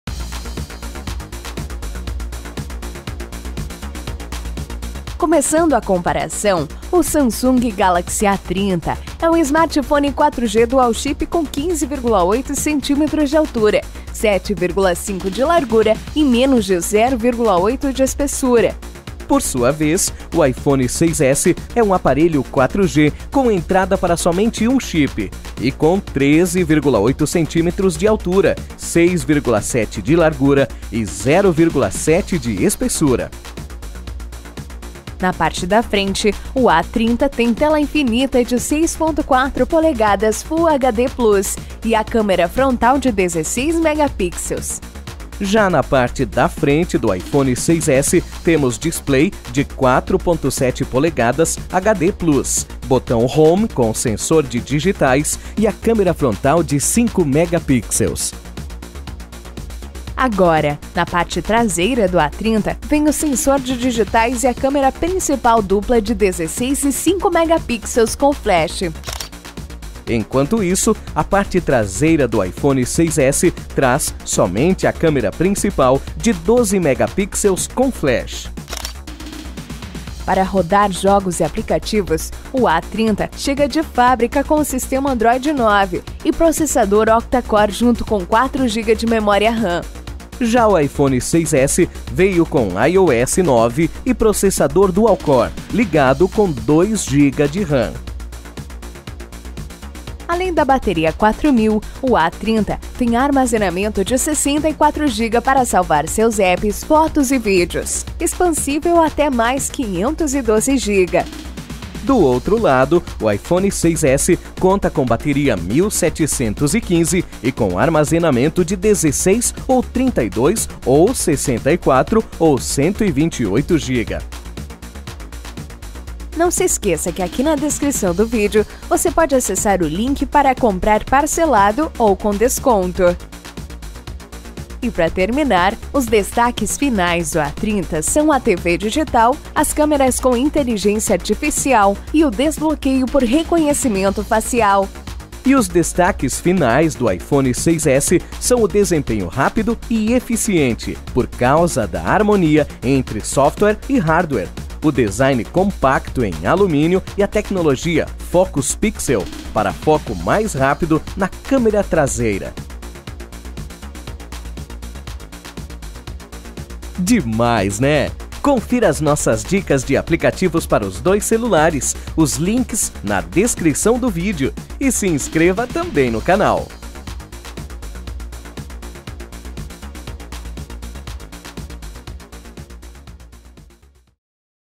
A30 vs iPhone 6s (Comparativo)
Confira a ficha técnica do Samsung Galaxy A30 e do Apple iPhone 6s, uma comparação de tamanho exclusiva (3D), e muito mais.